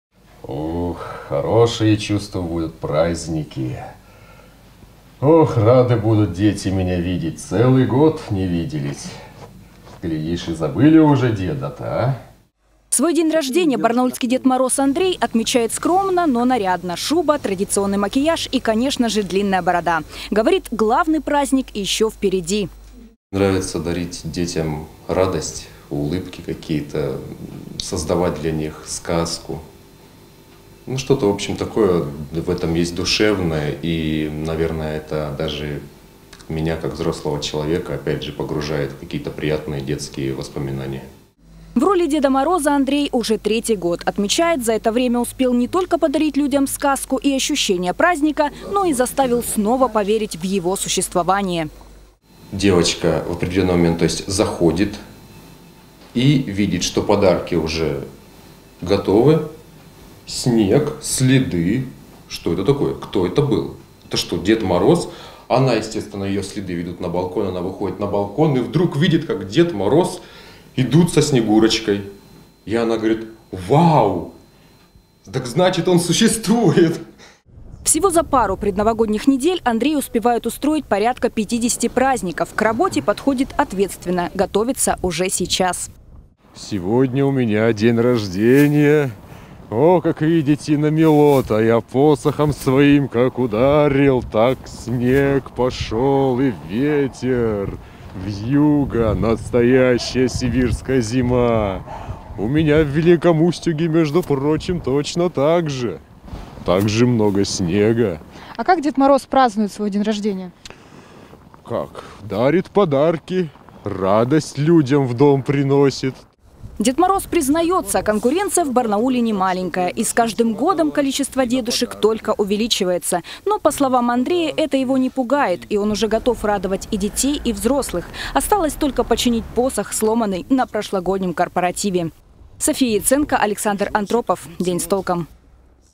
День рождения российского Деда Мороза. С этого дня, несмотря на календарь, наступает настоящая зима
Сегодня  день рождения российского  Деда Мороза. Считается, что именно с этого дня, несмотря на календарь, наступает настоящая зима. А значит, для Деда Мороза самое время доставать шубу и принимать заказы. Какой он, барнаульский Дед Мороз, и как готовится к праздникам – узнавала София Яценко